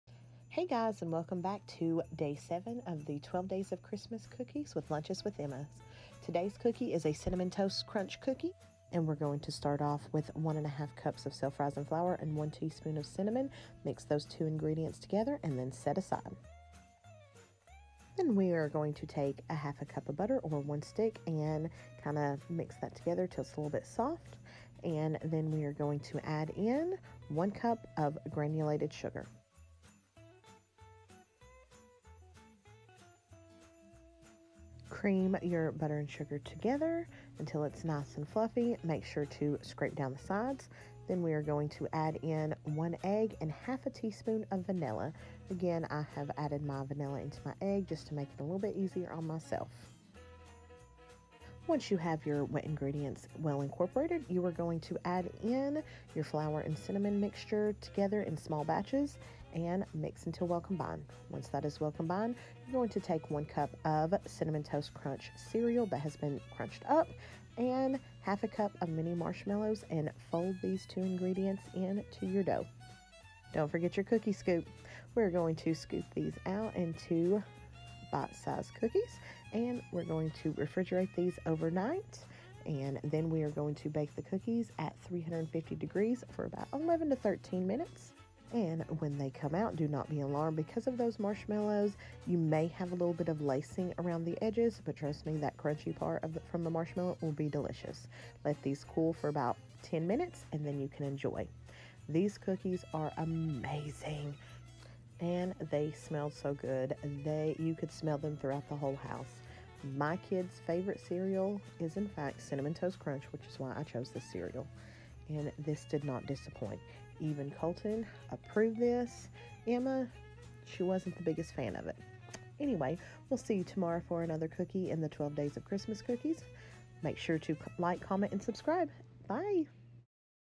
12 Days of Christmas Cookies Day 7; Cinnamon Toast Crunch Cookies
You always have to have a cinnamon cookie at Christmas time right? Or at least a warm spicy cookie. When I say my kids favorite cereal is Cinnamon Toast Crunch i mean they could eat it everyday if I let them 😂 these cookies didn’t disappoint and smelled up the whole house! Try these out!!

Random fact: I filmed baking 5 cookies yesterday. A day that I also toned someone’s hair with a red toner and it majorly stained my hands 🤣 next time I’ll wear gloves so it doesn’t look like I have something wrong with me 🤦🏻‍♀️

The original recipe;

To see daily photos and what she ate head over to Instagram at Luncheswithemma